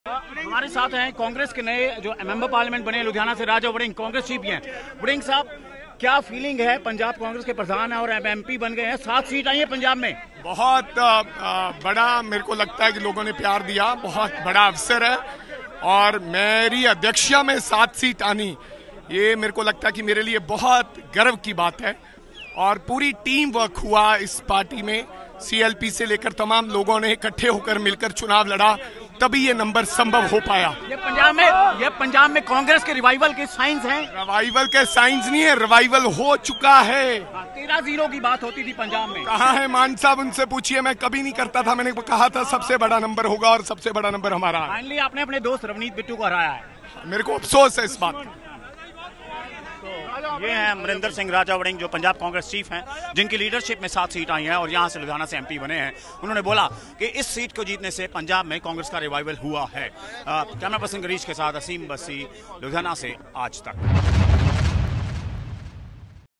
Election Result 2024: Ludhiana से Congress के प्रदेश अध्यक्ष Amrinder Singh Raja Warring जीते
पंजाब की 7 सीटों पर कांग्रेस की जीत, आजतक से बातचीत में जीत को पंजाब कांग्रेस अध्यक्ष अमरिंदर सिंह राजा वड़िंग ने बताया पंजाब में कांग्रेस का पुनरुद्धार।